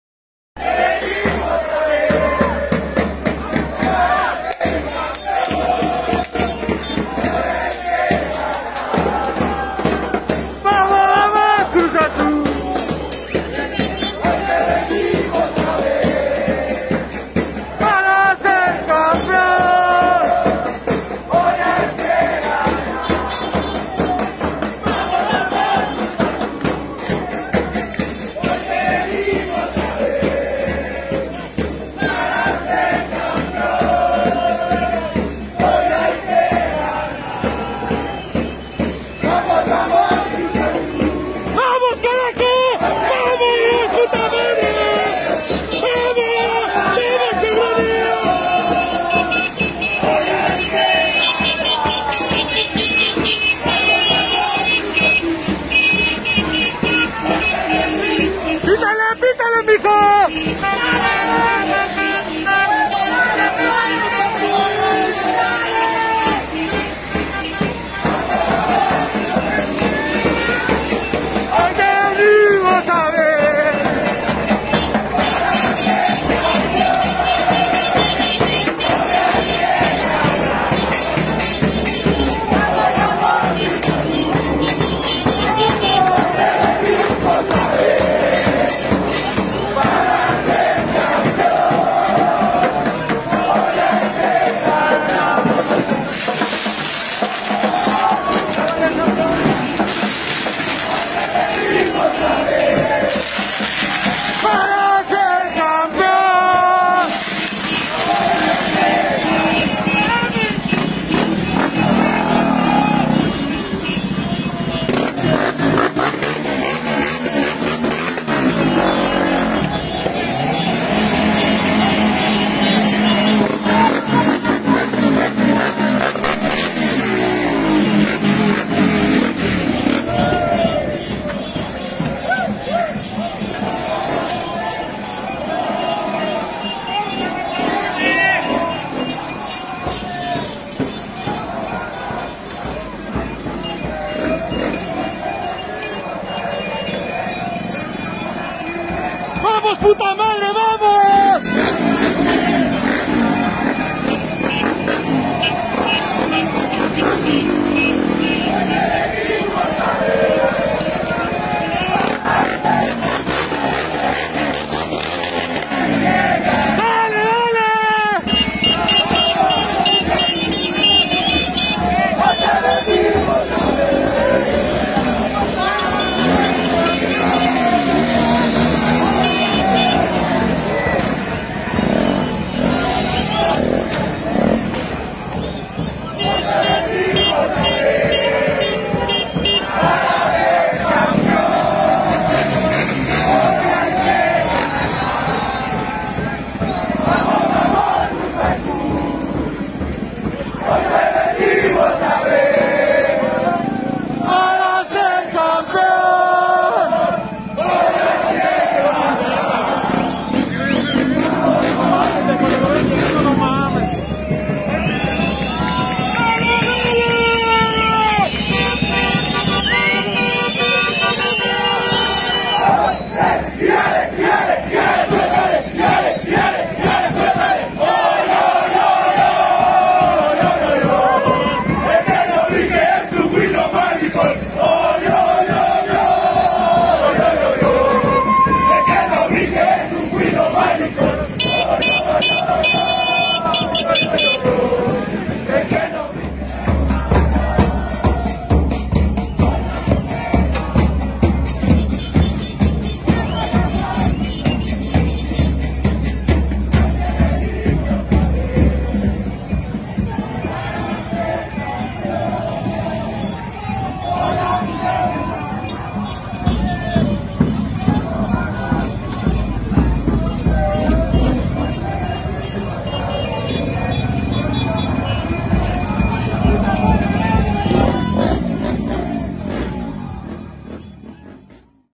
La Sangre Azul • Pasillo para recibir al equipo • Cruz Azul vs América
Si todos alentamos,
hoy la vuelta le damos... 💙🏆❤️

¡Eternamente Azul!